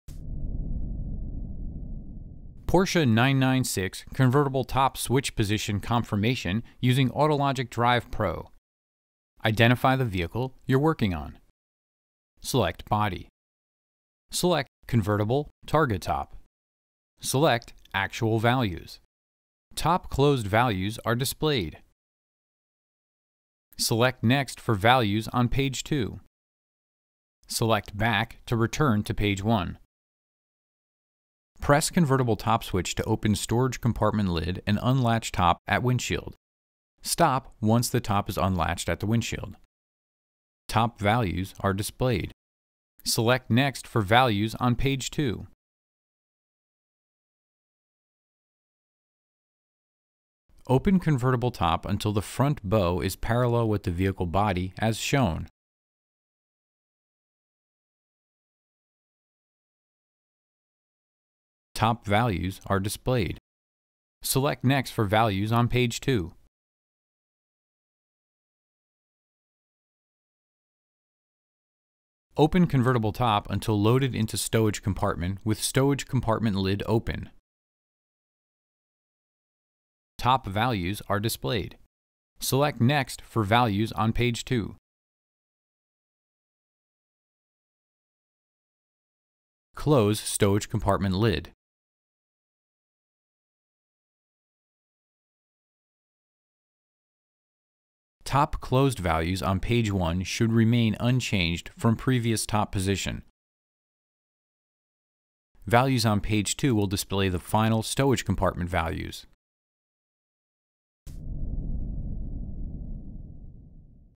Porsche 996 Convertible Top Switch Position Verification Using Autologic DrivePRO
In this video, the convertible top position switch nominal values are shown with the corresponding top positions.

For Porsche 996 Cabriolet models.